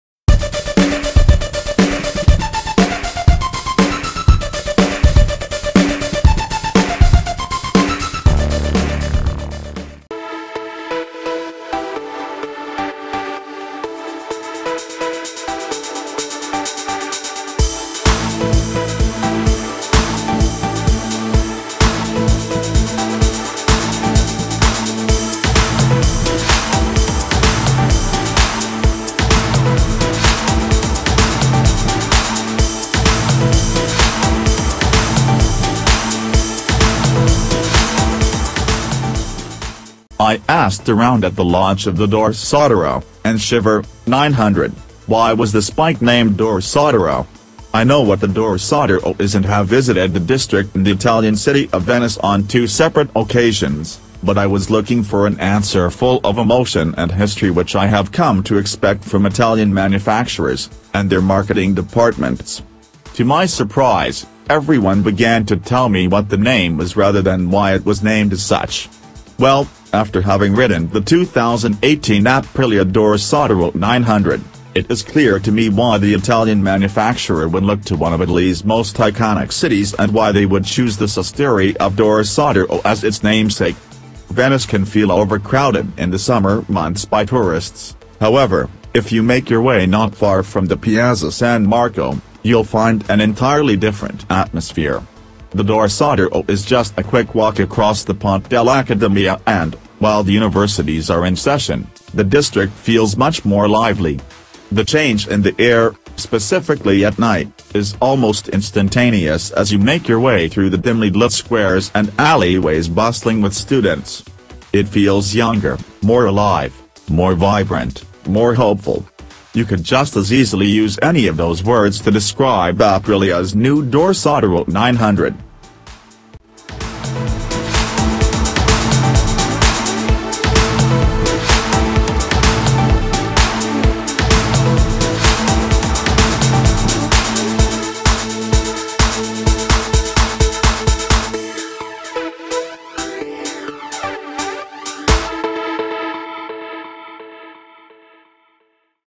[HOT NEWS] 2018 Aprilia Dorsoduro 900 First Ride Review
Well, after having ridden the 2018 Aprilia Dorsoduro 900, it is clear to me why the Italian manufacturer would look to one of Italy’s most iconic cities and why they would choose the sestieri of Dorsoduro as its namesake.